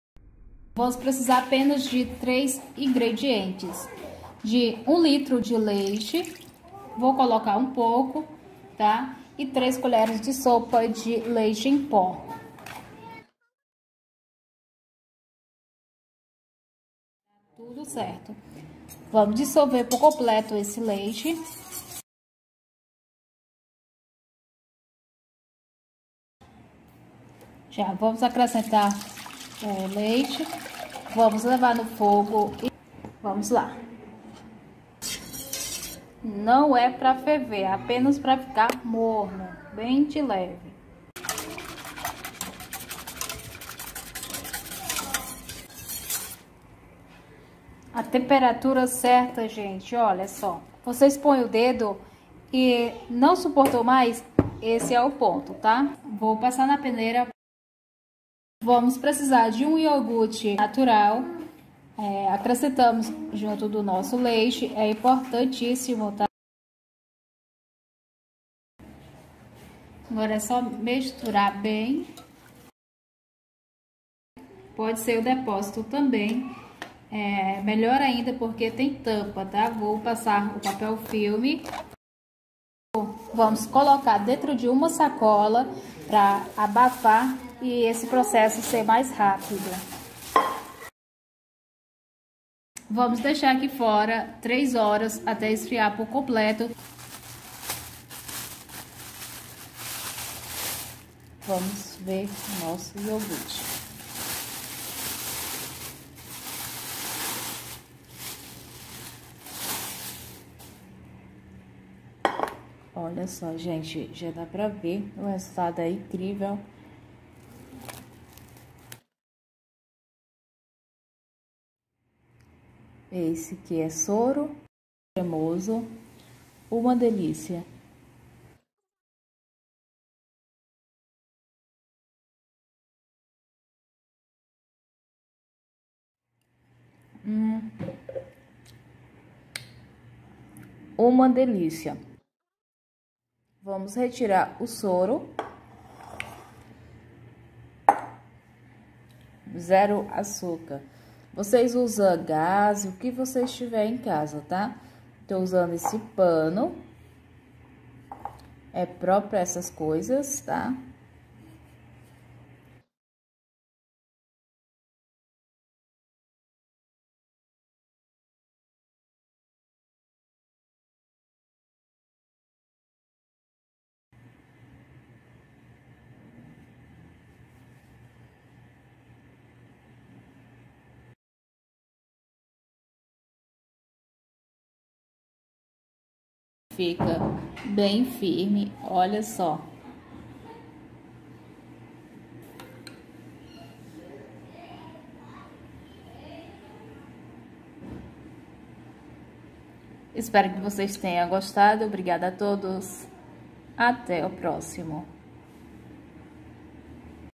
iogurte grego com 3 ingredientes sem gelatina sem açúcar econômico!!
Todos bem-vindo!

Fica incrível rende bastante, fiz com o leite de caixinha, se vocês tiver acesso o leite da vaca retirado, melhora ainda ficar idêntico😉!!
  Ingredientes
1 litro de leite
100G iogurte natural
3 colheres de sopa de leite em pó